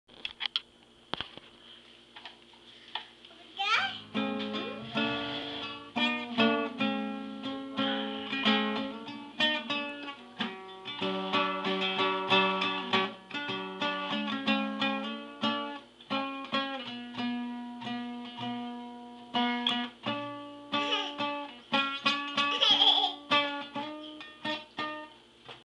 Alicja Helen plays her first Chords
Daddy and Alicja play chords together on a four stringed guitar.